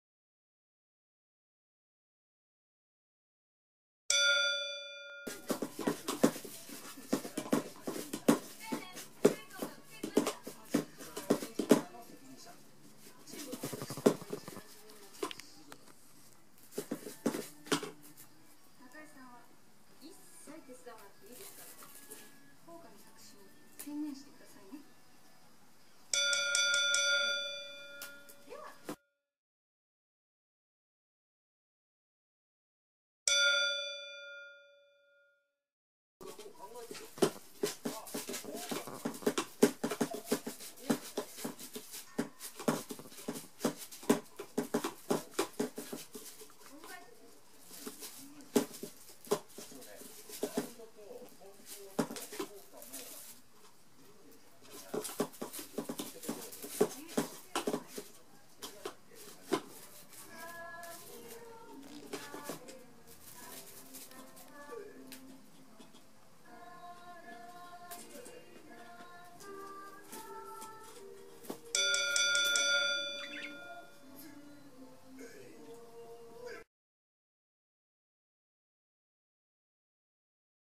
ポコちゃんvsじゃがりこ
ゴミ箱から出せば　ポコちゃんの勝ち。